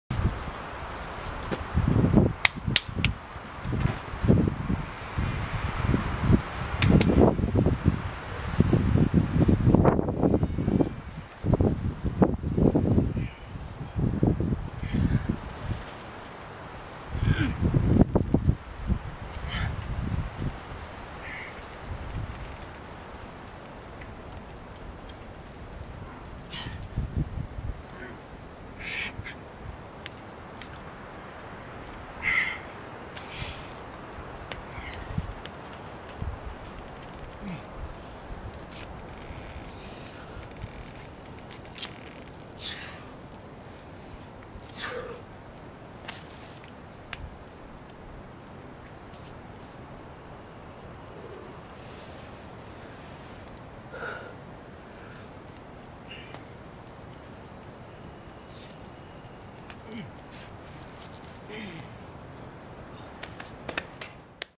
Cold Steel sit, 6C+ // Paradiset
🧗‍♂️ Climber:
Carl Nilsask

🗿 Problem:
Cold Steel sit, 6C+